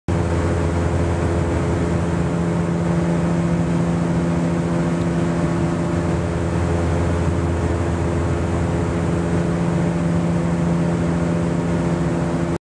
Mid shot of propeller and mountains. Mid shot of propelle...
Mid shot of propeller and mountains. Mid shot of propeller and mountains on April 05, 2013 in UNSPECIFIED, Federal Republic of Germany